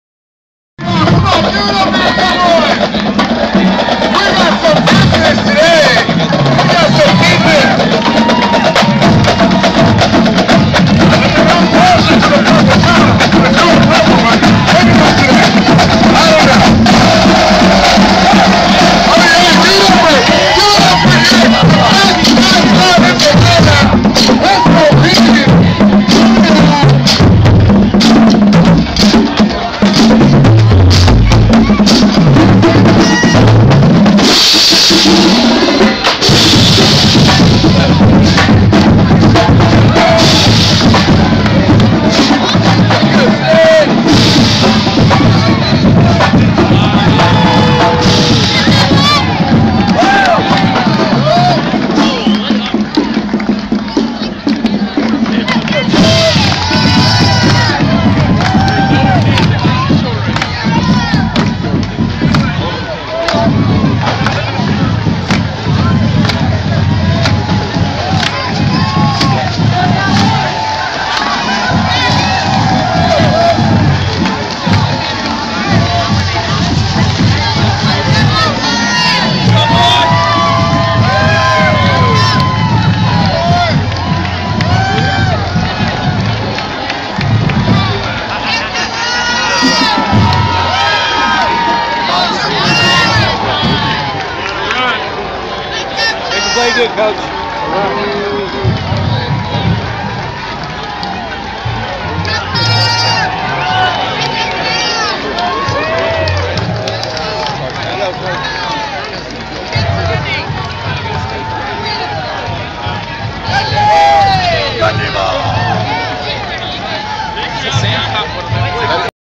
Oklahoma State The Walk
Oklahoma State team walks to the stadium vs TCU on 10-27-12